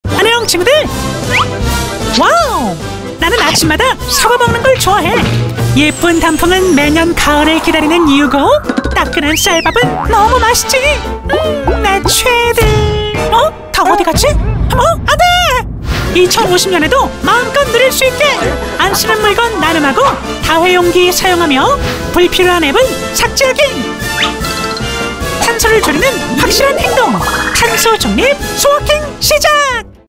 내 최애를 지키는 방법?
바뀌는 계절마다 자연이 선사하는
🌼🌳아름다운 풍경들.

아침마다 개운하게
한입 베어먹는🍎 빨간 사과.

김이 모락 모락~
뜨끈한 쌀밥까지!🍚

영상 속 미키가 꼽은
가장 좋아하는 것들인데요.

미키와 우리가 2050년에도
좋아하는 것들을 하기 위해선?

탄’소‘를 줄이는 ’확‘실한 ’행‘동을
실천해야 한다고 해요.

미키가 전하는 소확행 실천법,
같이 해보아요!👏

※ 본 영상은 제작 후 디즈니 코리아와
협의 후 송출됩니다.